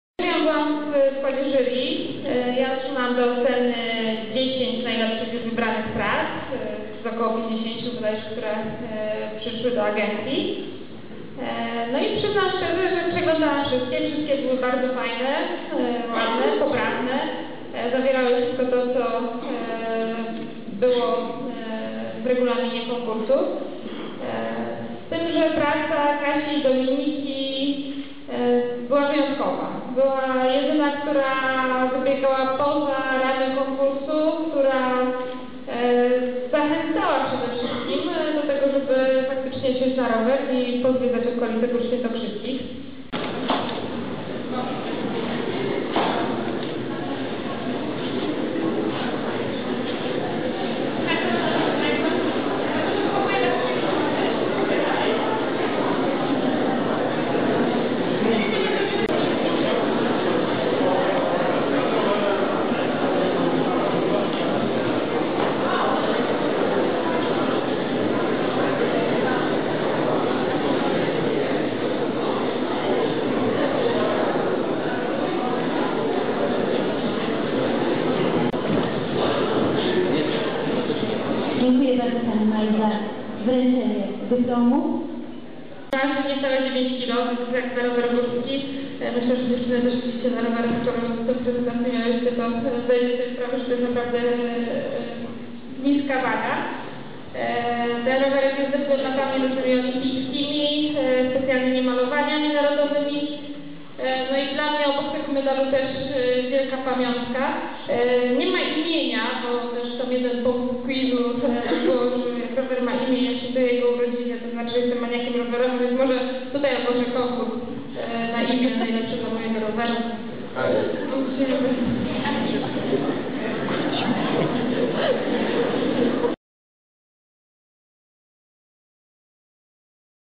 Maja Włoszczowska odwiedziła Kielce
Maja Włoszczowska, srebrna medalistka Igrzysk Olimpijskich w Pekinie w kolarstwie górskim gościła w piątek w Gimnazjum numer 9 w Kielcach. (Autor filmu: Dorota Kułaga)